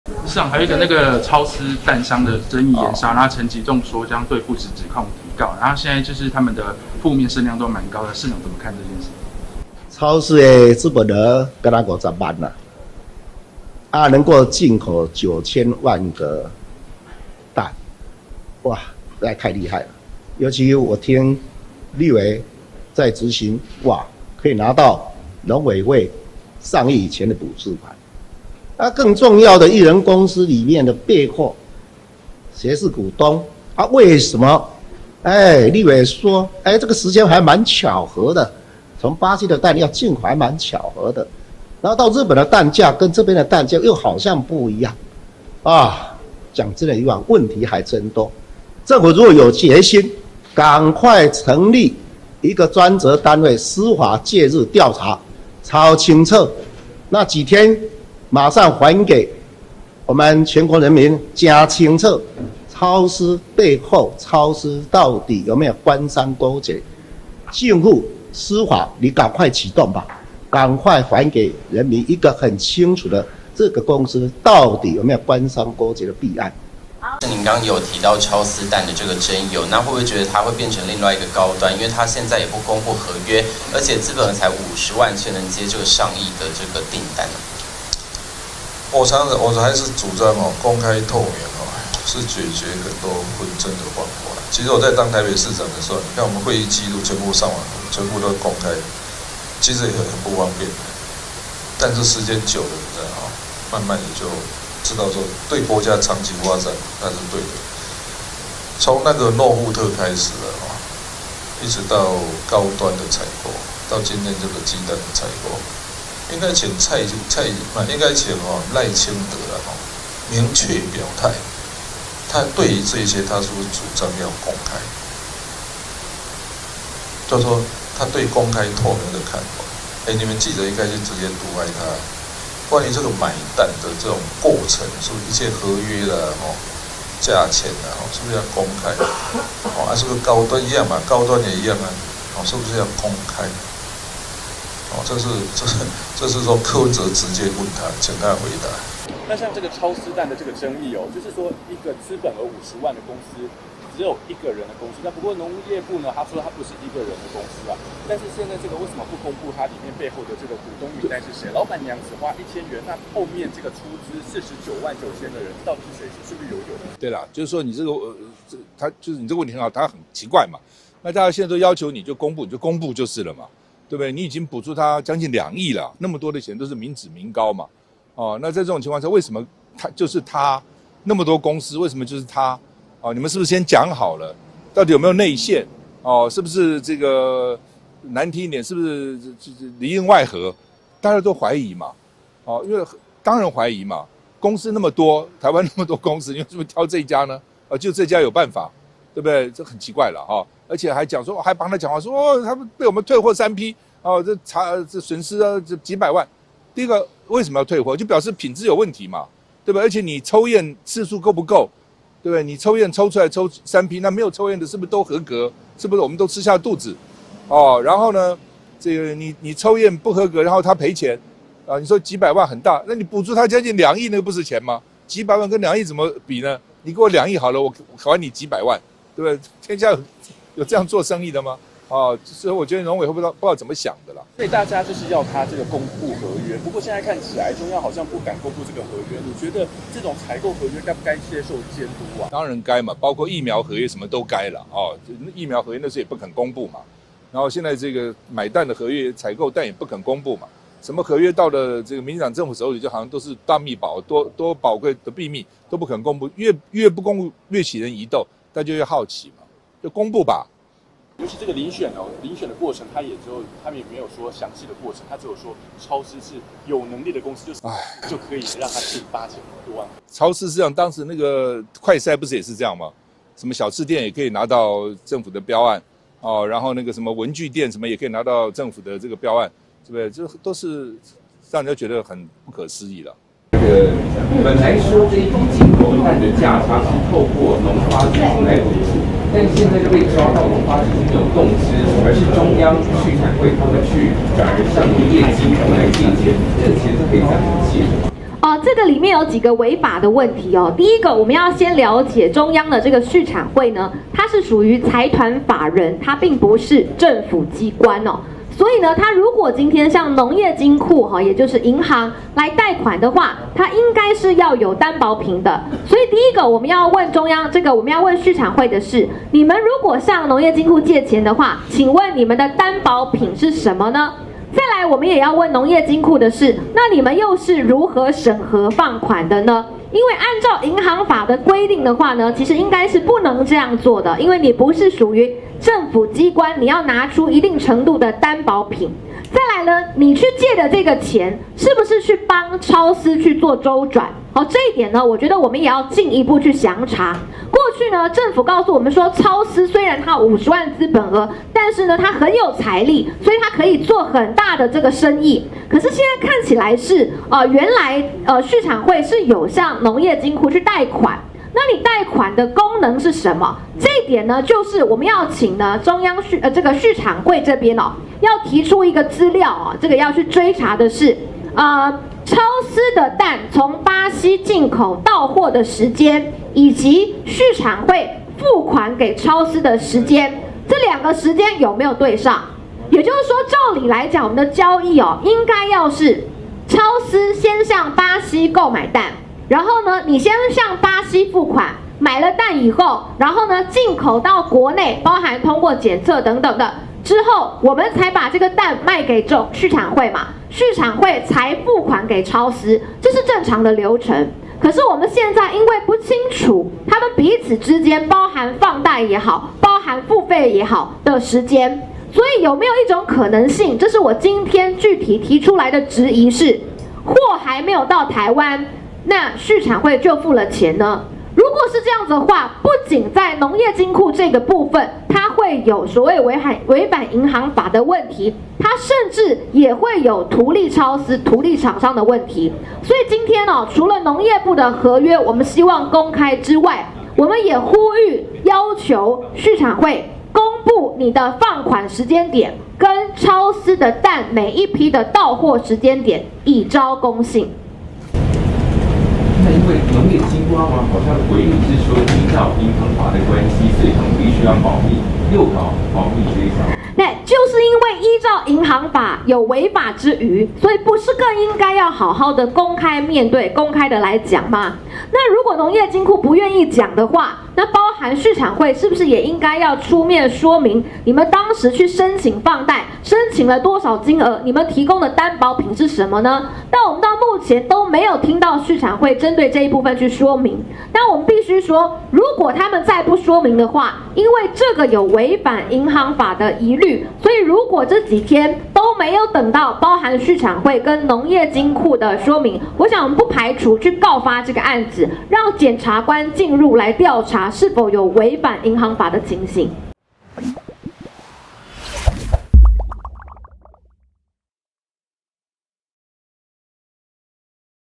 【全程字幕】陳吉仲為護超思蛋商揚言提告 侯友宜籲司法介入調查：還給人民真相
00:00 侯友宜回應超思進口蛋爭議
01:17 柯文哲回應超思進口蛋爭議
02:29 趙少康談超思進口蛋爭議
04:43 徐巧芯談超思進口蛋爭議